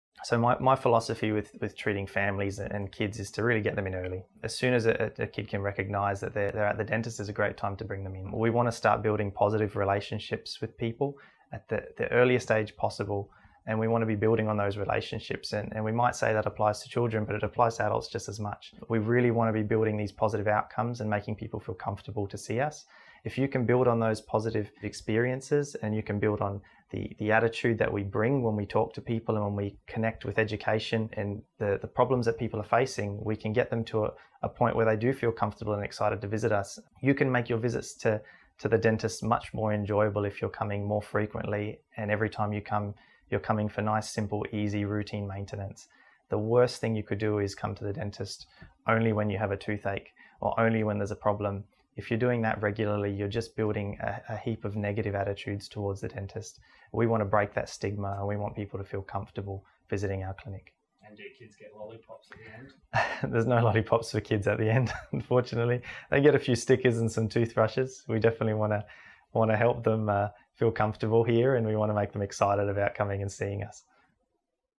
GoodLife Dental Interview 11
Series of short interviews for a local Darwin Business for the team at Realistic Views. Video and editing from MKH Visual Aspects Darwin.